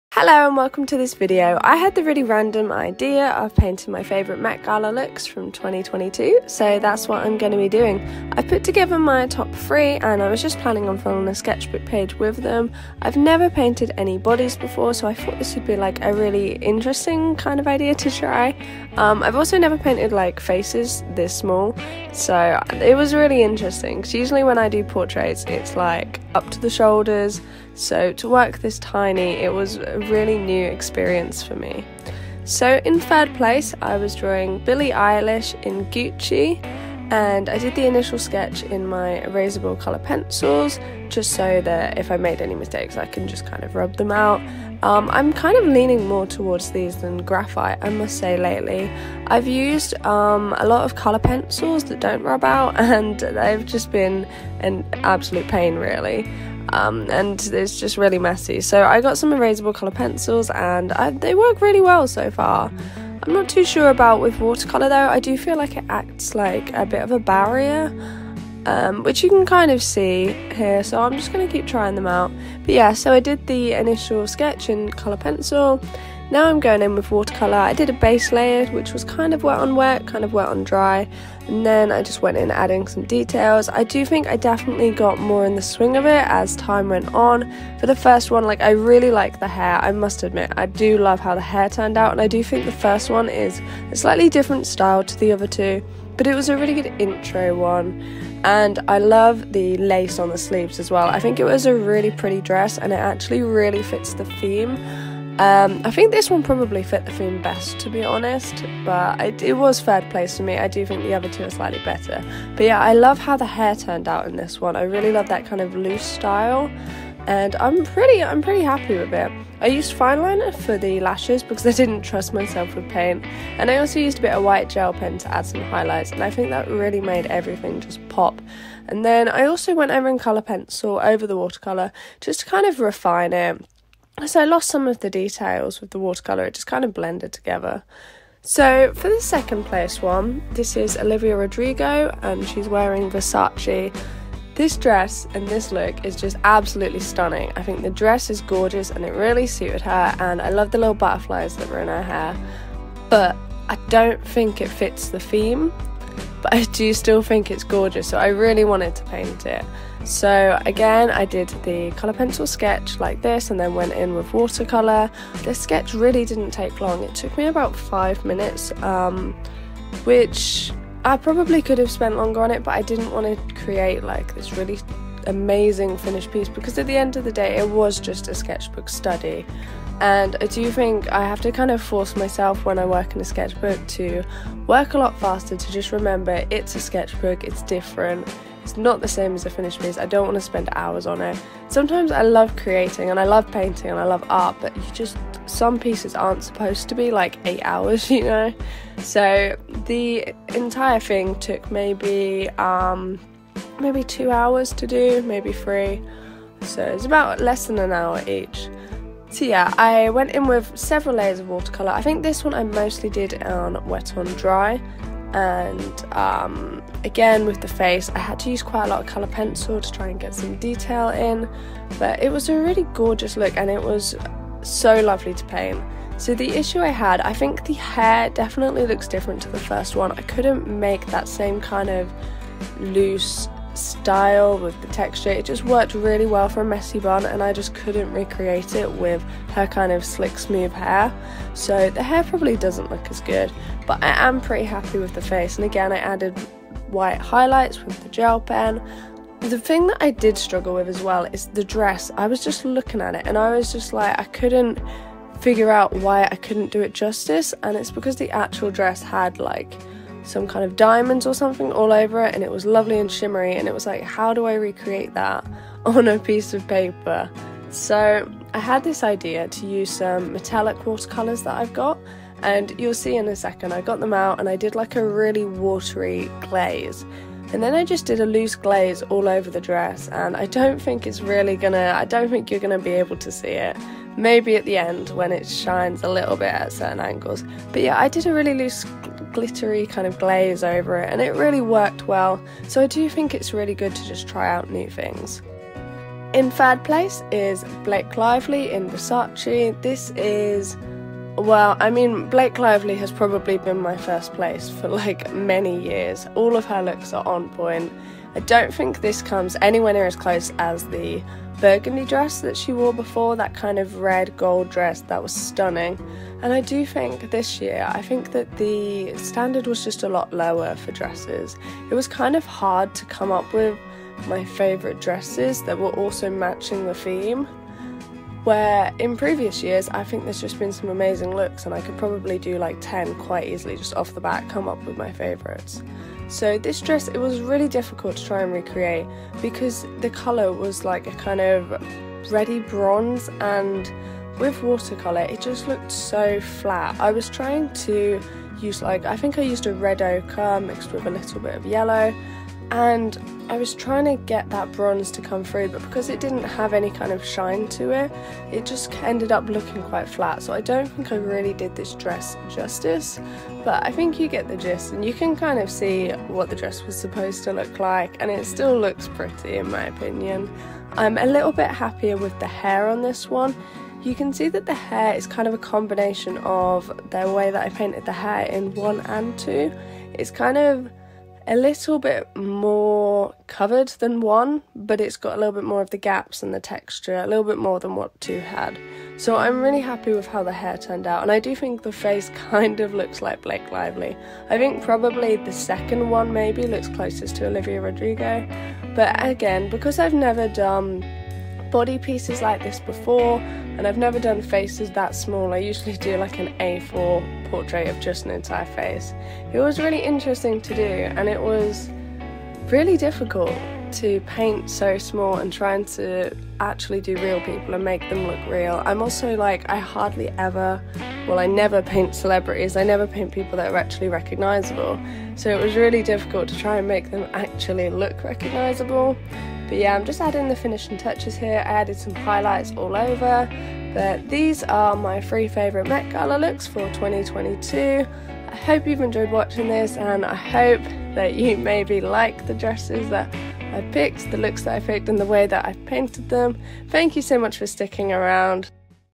Painting my TOP THREE Met Gala looks of all TIME
♡ Hey there! ♡ I'm Painting my TOP THREE Met Gala looks of all TIME in watercolour. Watch this timelapse of me speedpainting to fill a page in my sketchbook. It’s my first chatty video as I talk through my painting process so I hope you like it⭐️

✾ my fave art supplies ✾
✿ brushes ✿
synthetic quill paintbrush
princeton paintbrush
✿ paint ✿
himi jelly gouache
✿ paper ✿
arches block
etchr sketchbook
✿ equipment ✿
fineliners

❈ chapters ❈
00:00 dress one
02:14 dress two
04:44 dress three